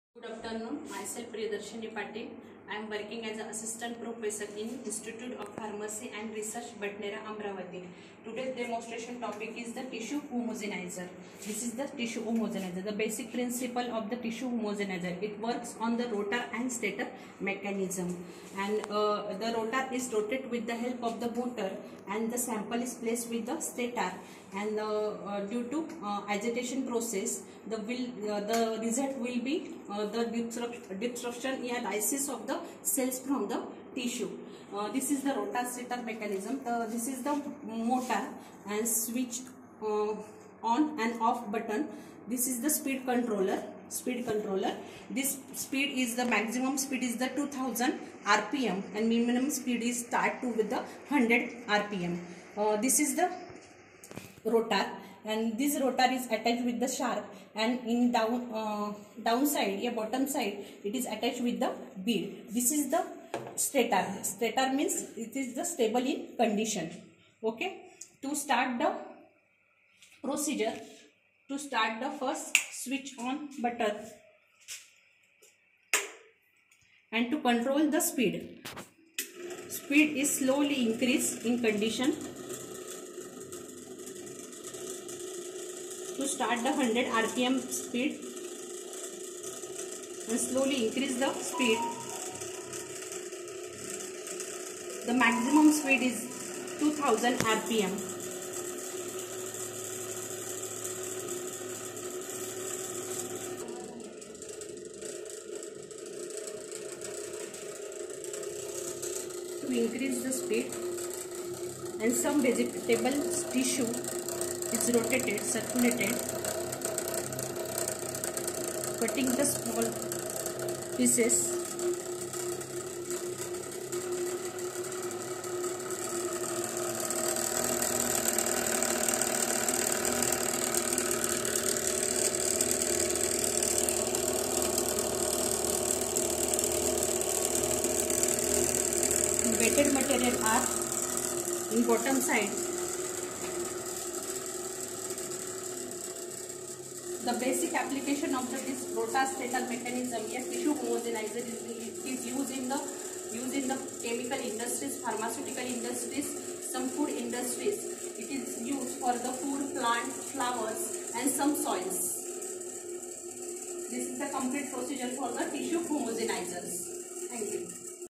Principle, construction and working of Tissue homogeniser
Principle, construction and working of Tissue homogeniser by Ms. Priyadarshini Patil, Assistant Professor, Institute of Pharmacy and Research, Badnera